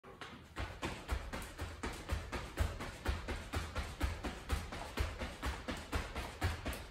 In and Outs
- Start in an athletic (base) position
- Think about head staying at same level the whole time
-Quickly punch feet to outside of hip width and back under you
-Get arms involved